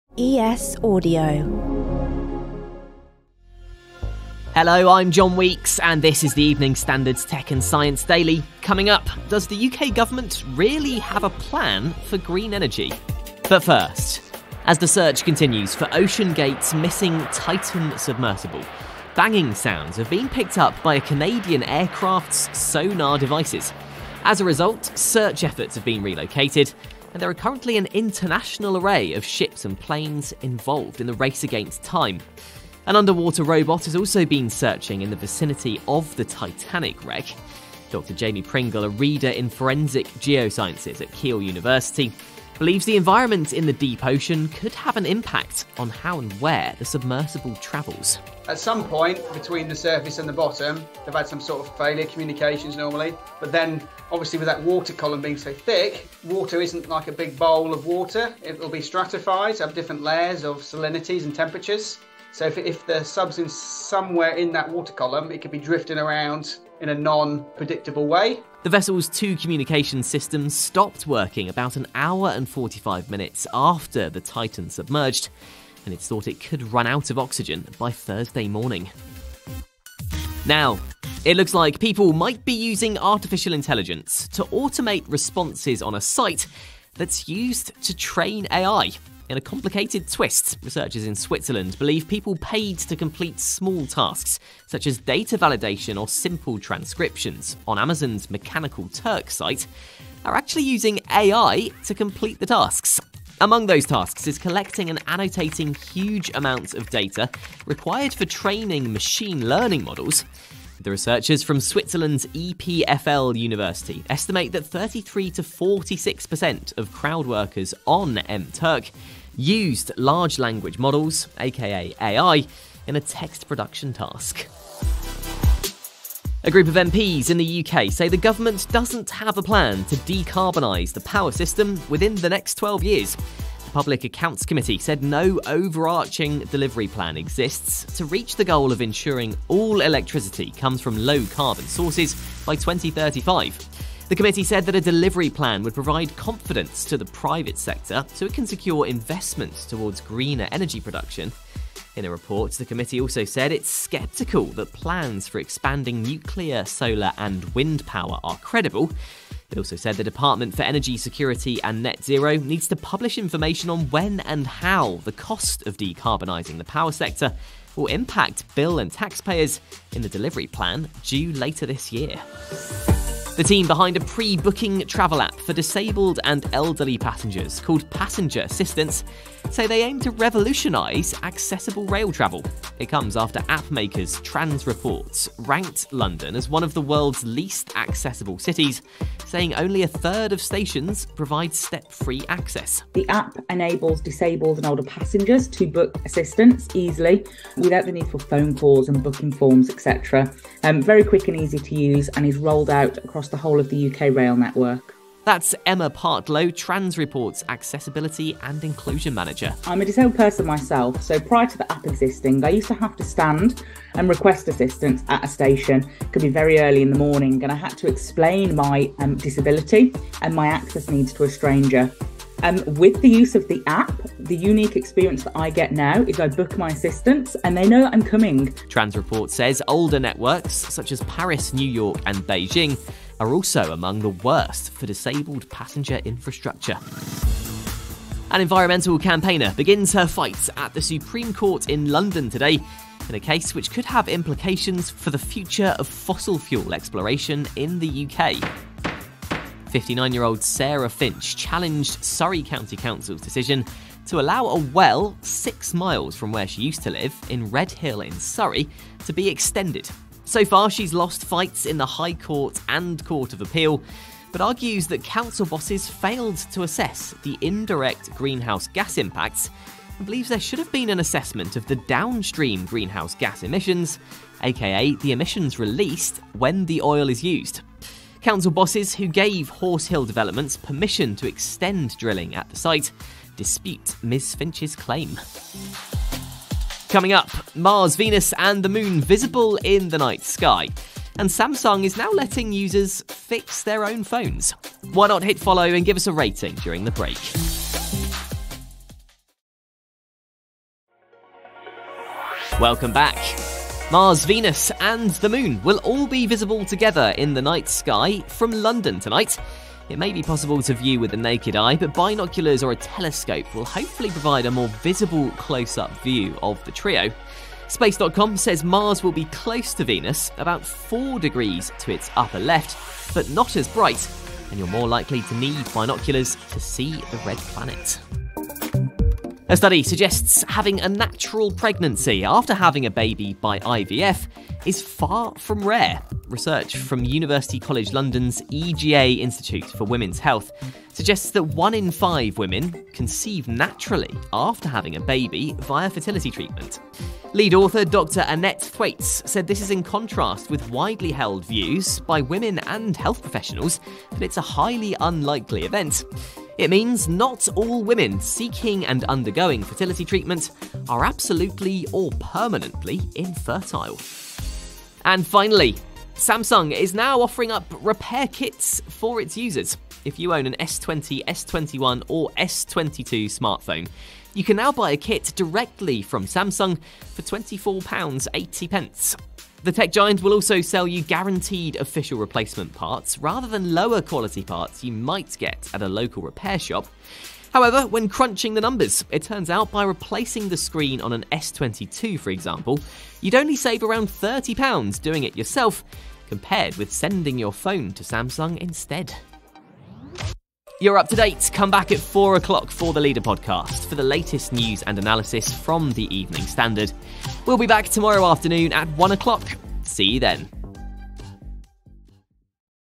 Missing Titanic Sub: ‘Banging’ sounds heard by sonar devices
Canadian aircraft’s sonar detects underwater sounds in search for the OceanGate Titan sub. How people might be using AI to automate responses on a site that pays them to train AI. Does the UK government really have a plan for green energy? In this episode:

Transreport's Emma Partlow explains Citymapper-style rail app to revolutionise disabled travel Environmental campaigner set for Supreme Court oil drilling fight
Mars, Venus and the Moon to be visible from London over summer solstice
One in five women ‘conceive naturally after having baby via fertility treatment
’Samsung kits let you repair your own phone from home